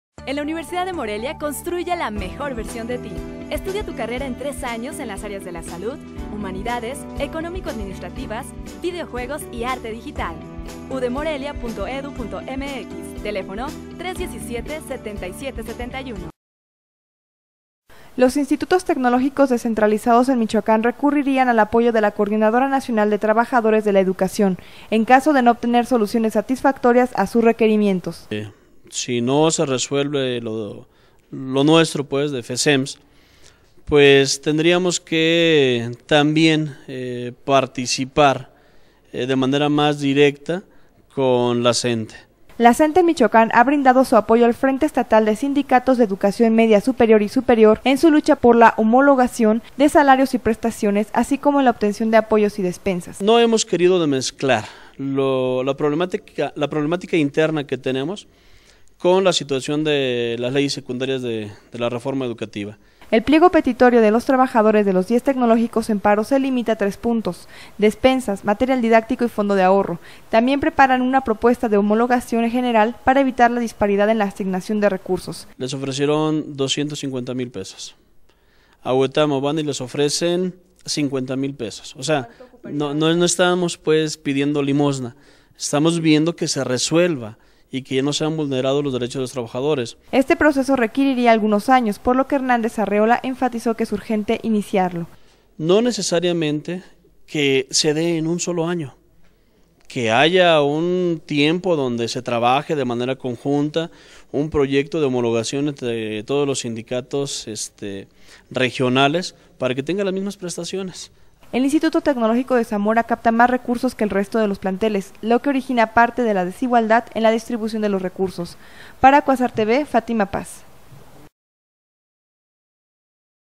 Exigen Tecnológicos homologación de gasto y prestaciones
Los institutos tecnológicos descentralizados en Michoacán recurrirían al apoyo de la Coordinadora Nacional de Trabajadores de la Educación (CNTE), en caso de no obtener soluciones satisfactorias a sus requerimientos...